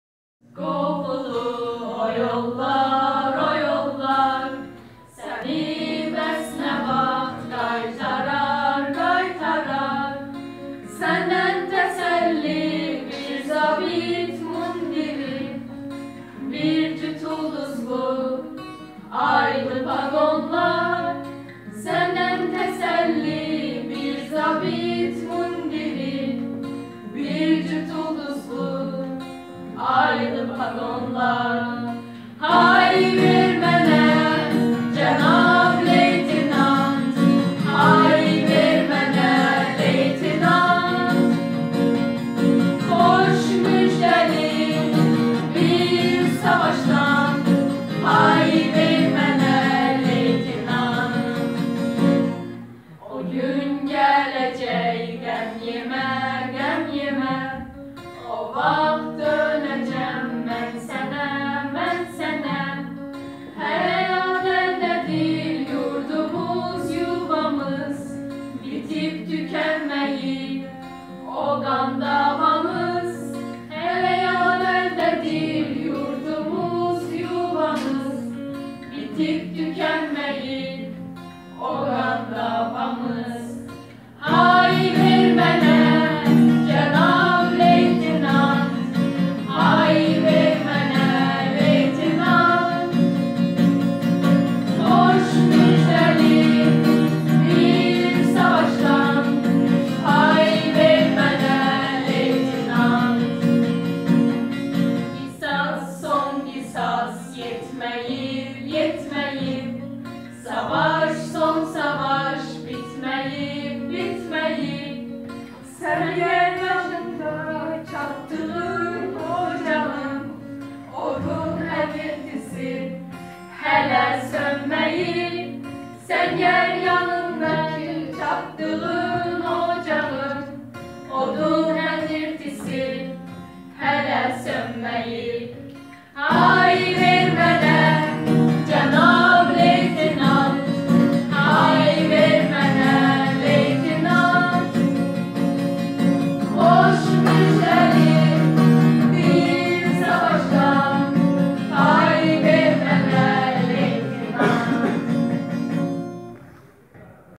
Vətənpərvər Xaçmaz məktəblilərinin ifası
Xaçmaz Rayon Mehman Babayev adına 1 nömrəli tam orta məktəbin 10-cu 11-ci sinif şagirdlərinin vətənpərvərlik mövzusunda çıxışları.